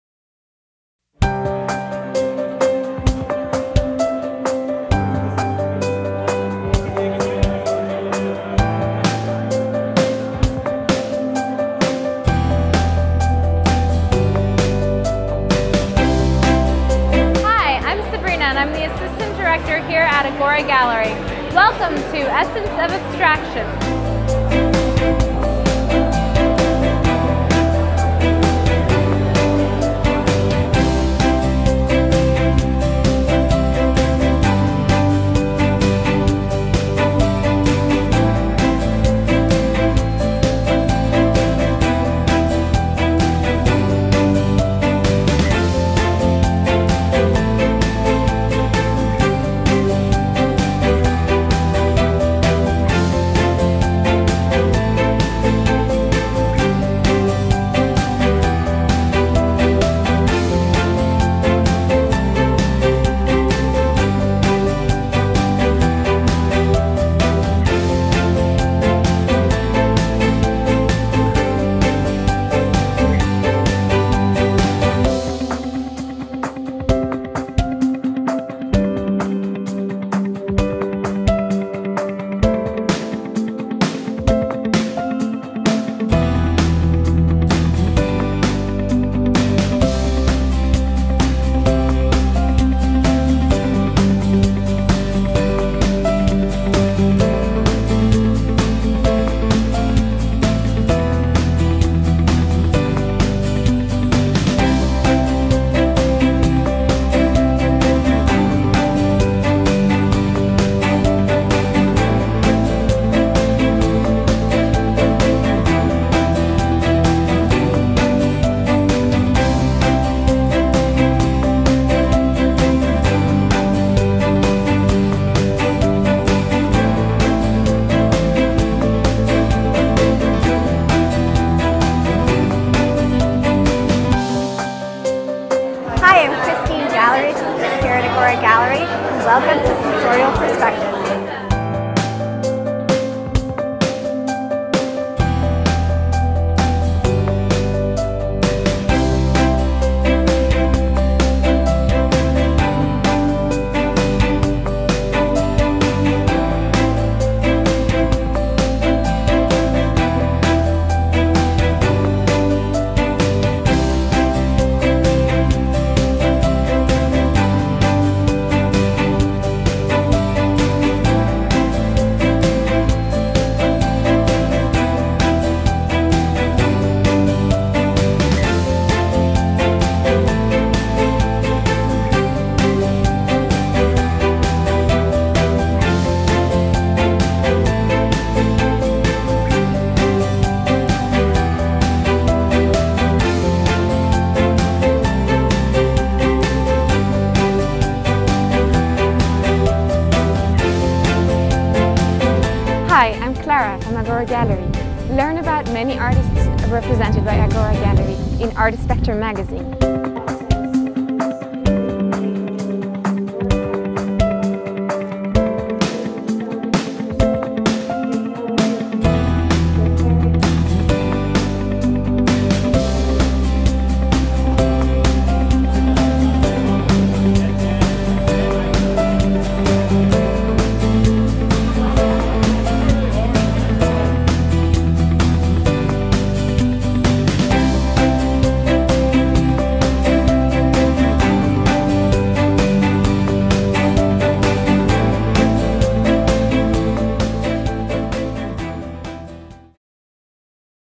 Agora Gallery, Chelsea, NYC, Art Gallery Video. Opening Reception March 14, 2013
Agora Gallery is proud to present "Sensorial Perspectives" and "The Essence of Abstraction" currently on display through March 22, 2013. "